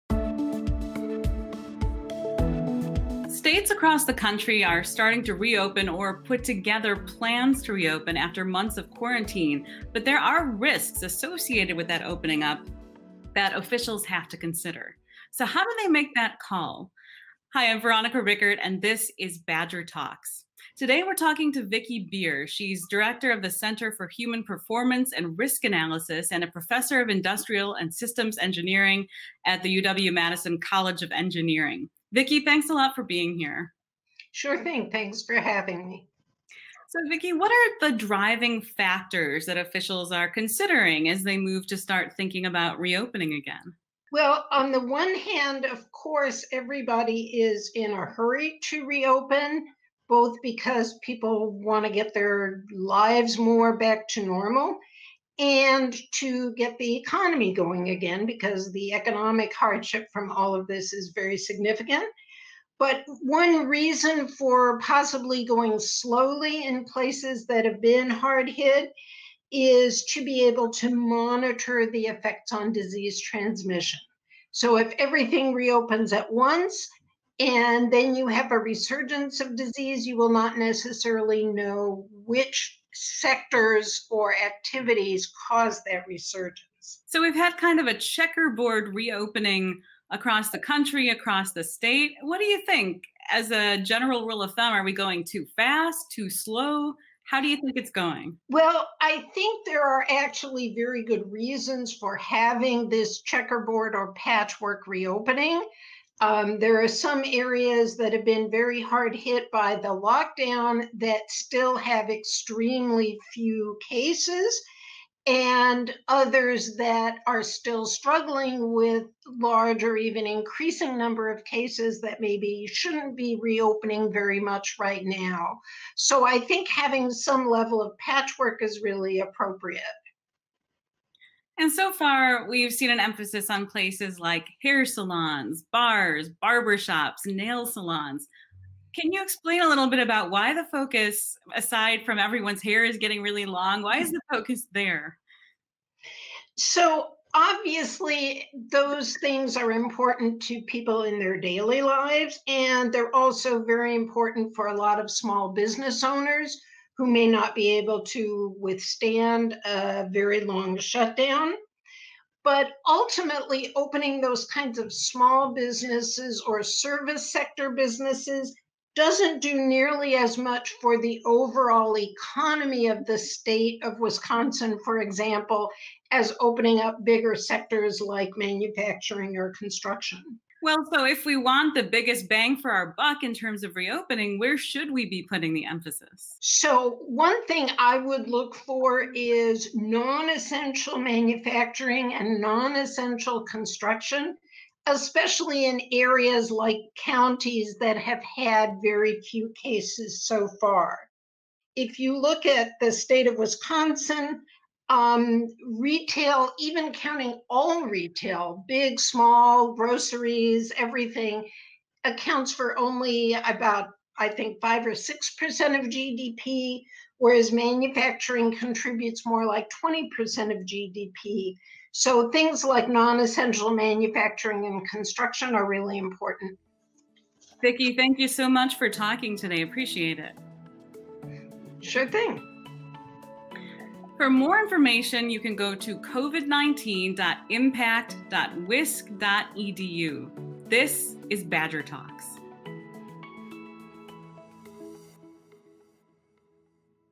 Badger Talks: The Risks of Reopening
Risk analyst Vicki Bier, a professor at the UW-Madison College of Engineering, talks about the decision-making process that goes into how to reopen a community after quarantine.